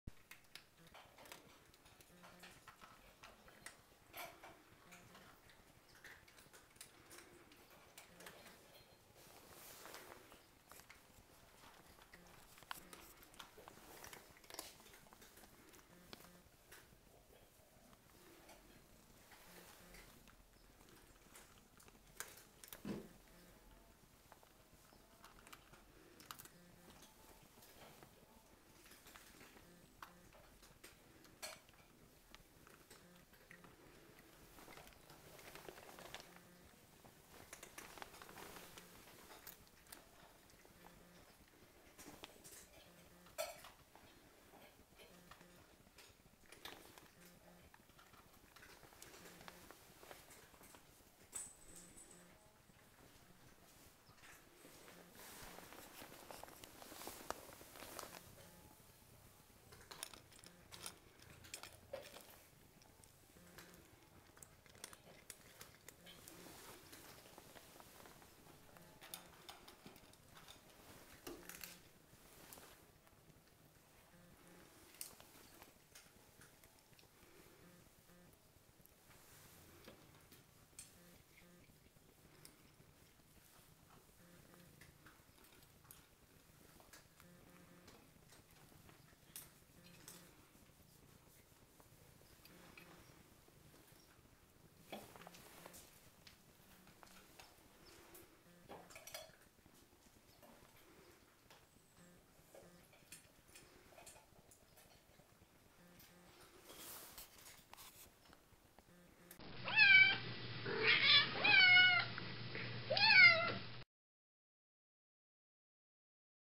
Dinner time in our cage-free cat cottage
Watch as some of the Sanctuary's rescued cats chow down at feeding time. They like a little lovin' for seasoning, so whenever we can, we pet them as they eat, so that they know what pretty kitties they are.